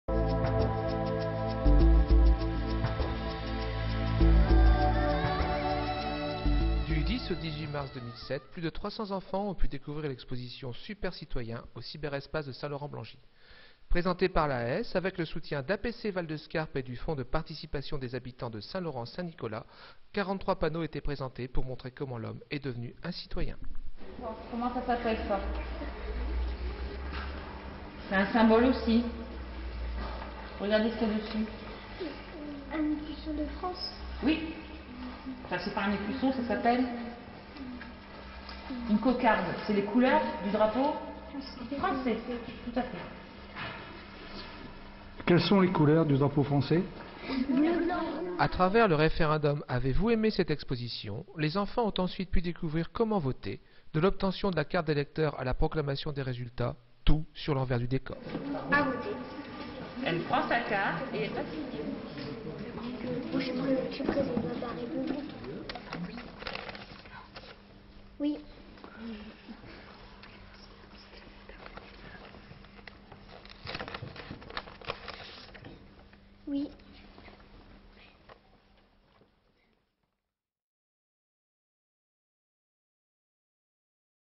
REPORTAGE SUPERCITOYENS
EXPOSITION SUR LA CITOYENNETE

Reportage sur l'exposition Super Citoyens à l'Espace Léo Lagrange de St-Laurent-Blangy